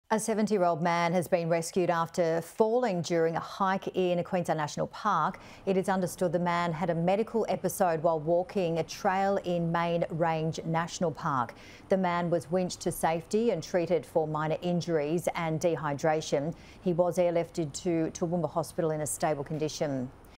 Hiker rescued from Queensland national park after suffering fall
A 70-year-old man has been rescued after falling during a hike in a Queensland national park.

It's understood the man had a medical episode while walking a trail in Main Range National Park.

The man was winched to safety and treated for minor injuries and dehydration.

He was airlifted to Toowoomba Hospital in a stable condition.